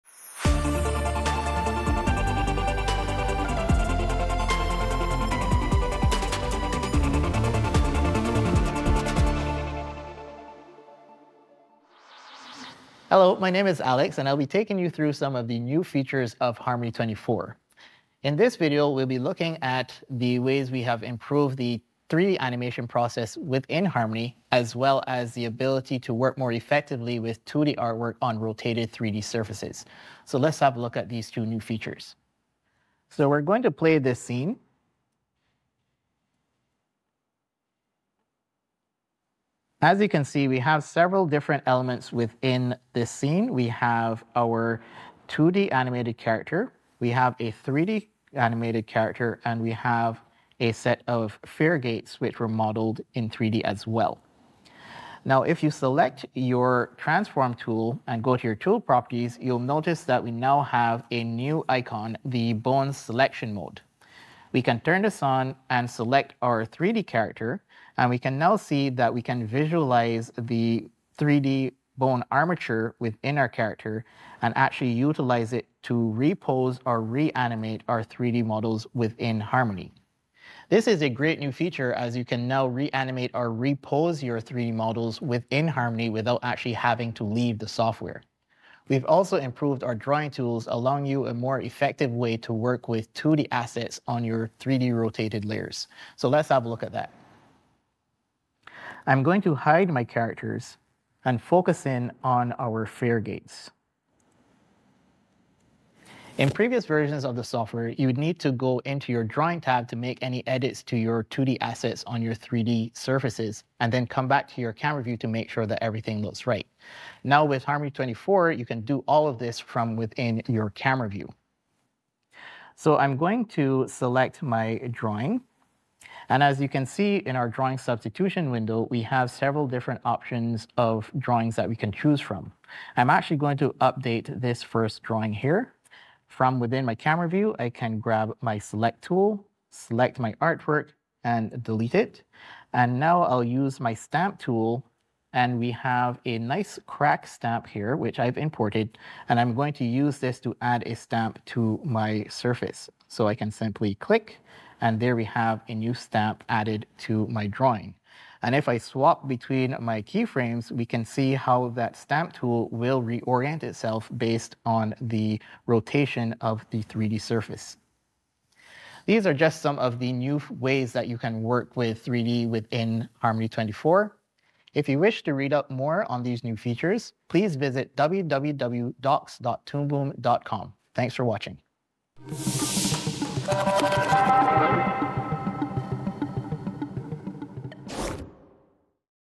Harmony 24 - 3D - Características Creativas I Subtítulos en español
En este vídeo, veremos cómo hemos mejorado el proceso de animación 3D en Harmony y la capacidad de trabajar más eficazmente con ilustraciones 2D en superficies 3D rotadas.

Para más información sobre Toon Boom Harmony, visite: toonboom.com y docs.toonboom.com.

► Obtenga más información sobre las novedades de Toon Boom Harmony. Realice un recorrido por las funciones más recientes y descargue una versión de prueba gratuita de 21 días: